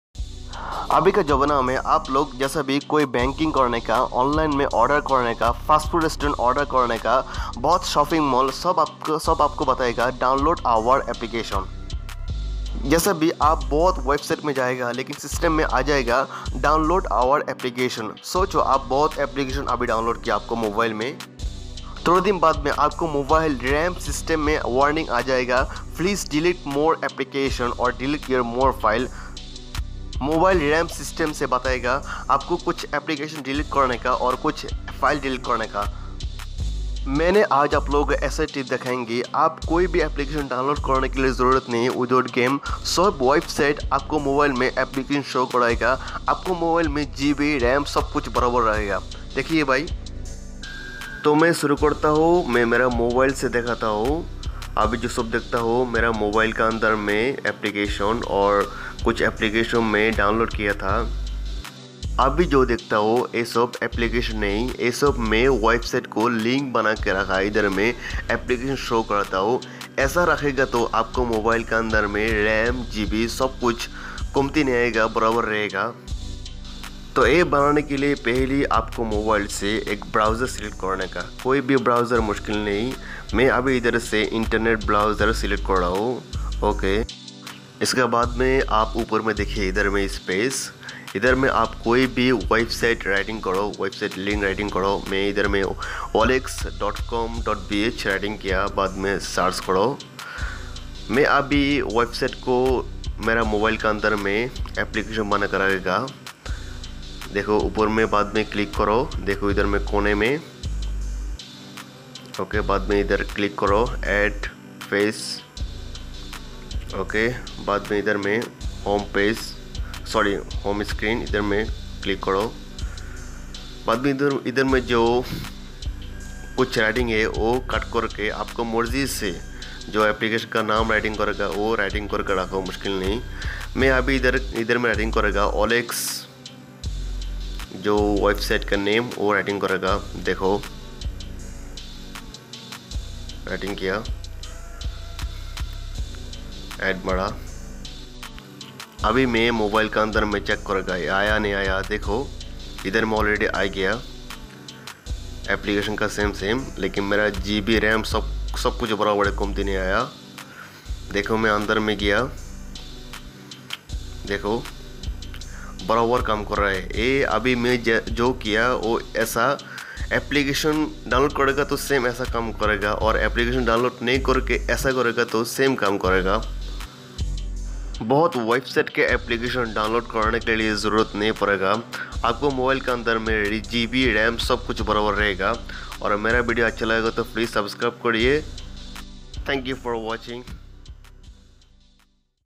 Wedside applications without download solve you problem || save you ram GB in Hindi
this video i am learning many wideside they say you  Download our applications,  my trip without download applications  you can solve you problem,  this is how I doing,  first I am go to wedside inside than i select icon for saving link,  from Icon select home screen for saving,  I am not following any copyright,  music from youtube free music,  voice my salf, and I add also shopping mall video,  I salf create video, video language in hindi